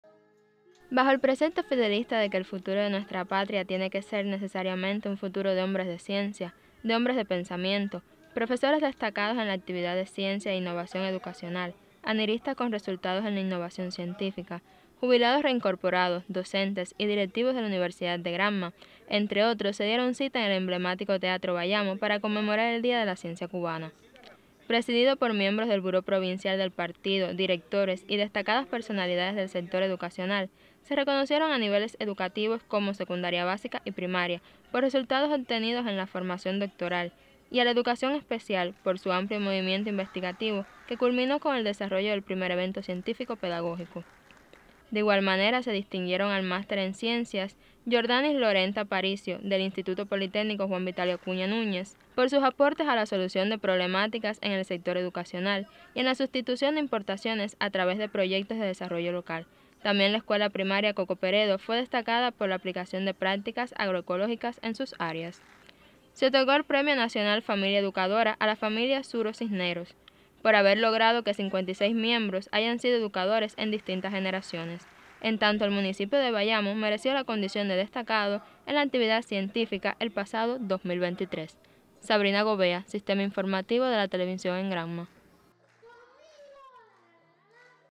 Reconocen a destacados investigadores en el día de la ciencia cubana.
En el Teatro Bayamo se celebró el acto por el Día de la Ciencia Cubana.

En la cita se premiaron a personalidades del sector y centros educacionales que se destacaron por su quehacer en el ámbito científico, así como por sus proyectos en búsqueda de sustituir  importaciones y potenciar el desarrollo local.